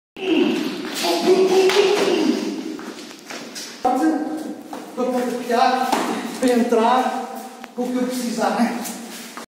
Combat Survival FMS/PBIT KJN Pinto & Inst. Oliveira
Kickboxing and Kicking action
This video was only done for Demonstration purposes, always train under a certified Instructor an authorized by your Doctor.
Pinto Martial Arts Greenville/Simpsonville South Carolina
FMS is derived and License by WHRDA